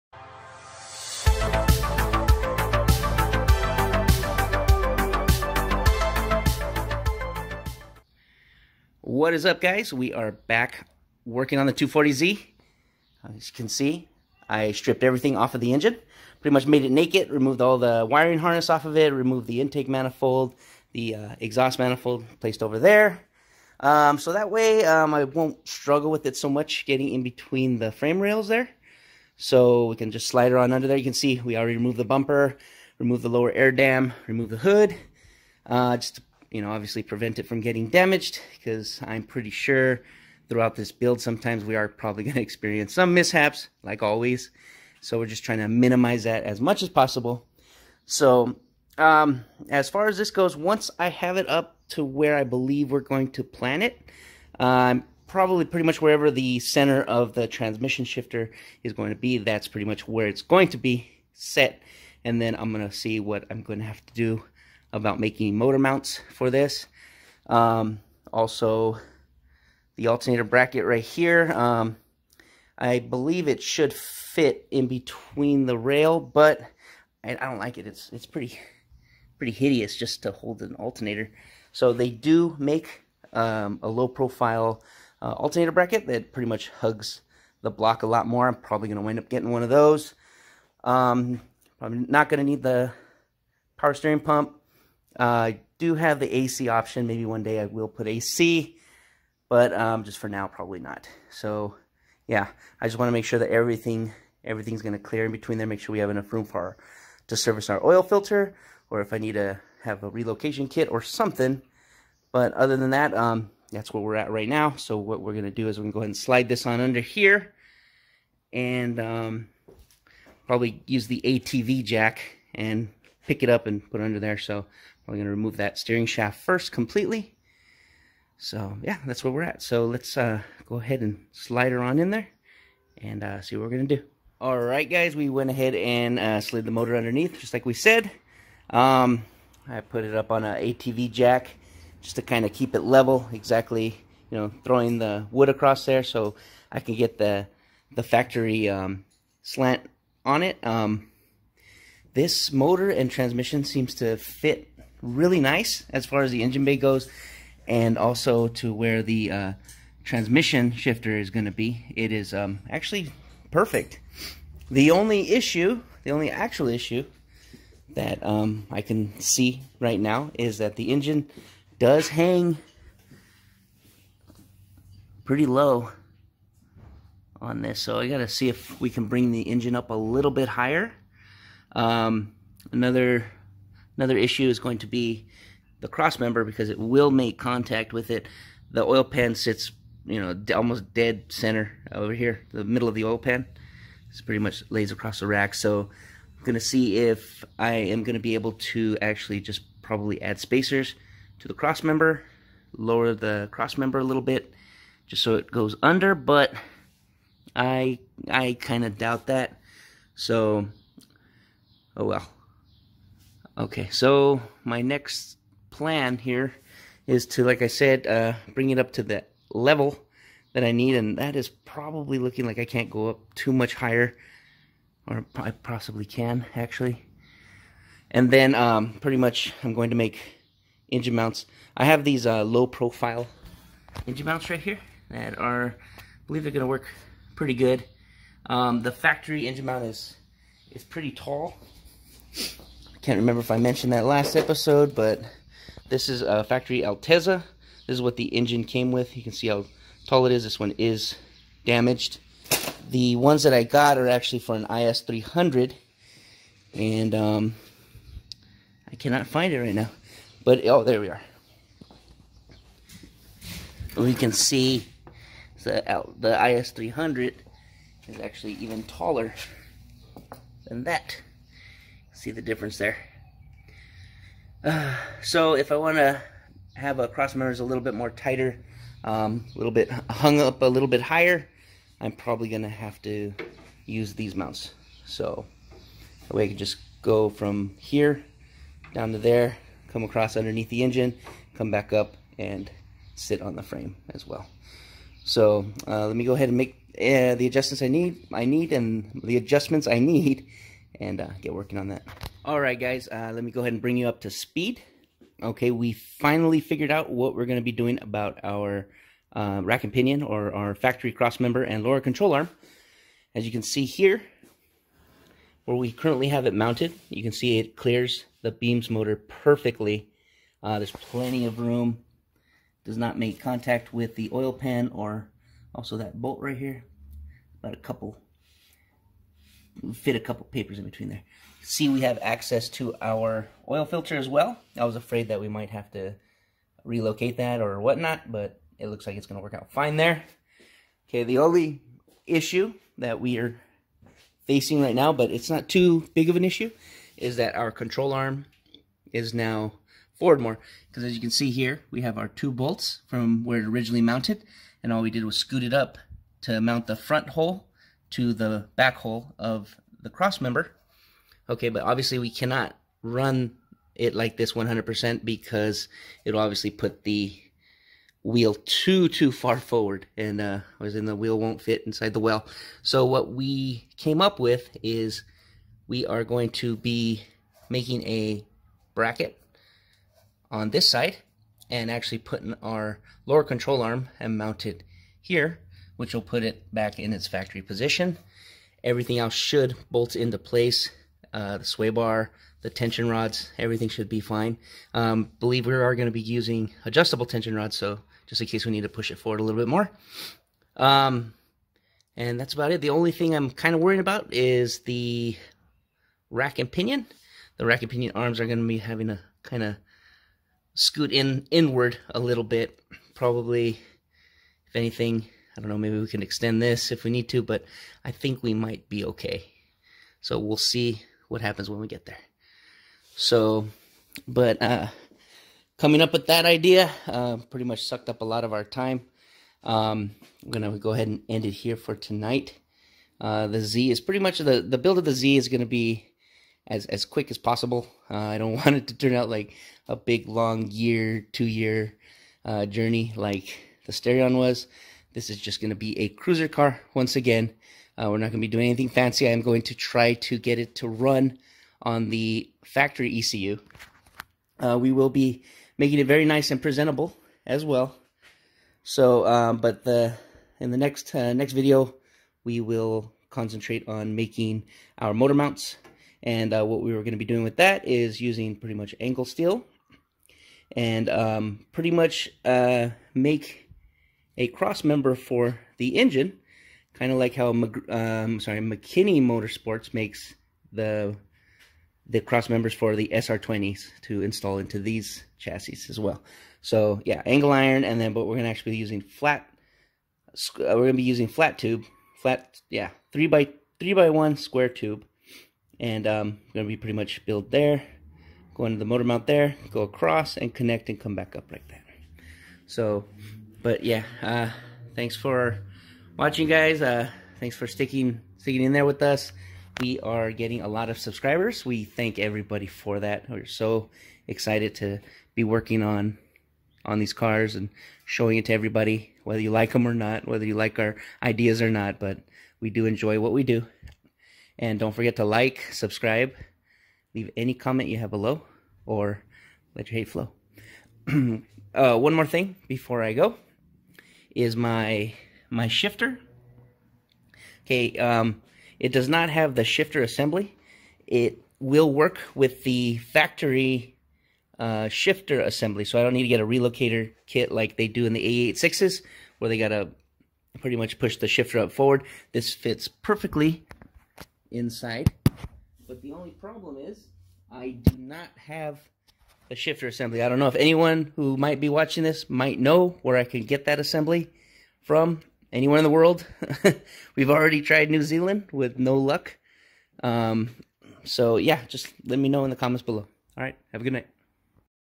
3SGE Beams Swapped 240z Ep.2
Swapping this motor is a little more challenging than I thought. We get the engine into position and come up with a plan for the steering.